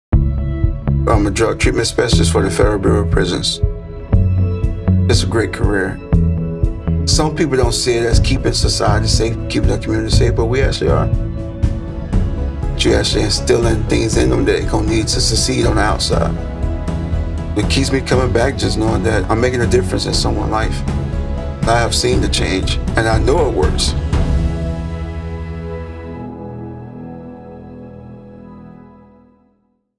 The Inside Story | Drug Treatment Specialist | BOP | :30 Seconds
An inside look at treating substance use in prison.